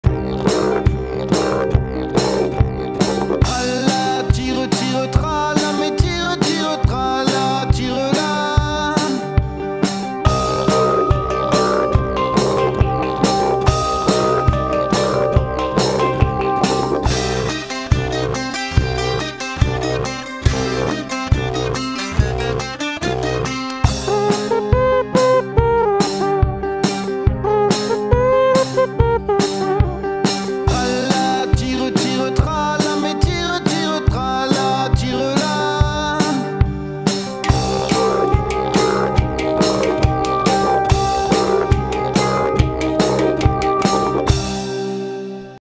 Déambulation - Homme-orchestre - Apach
Déambulation - Homme-orchestre
Avec chariot équipé d'une sono, didgeridoo, handpan, mandole, batterie dans le dos.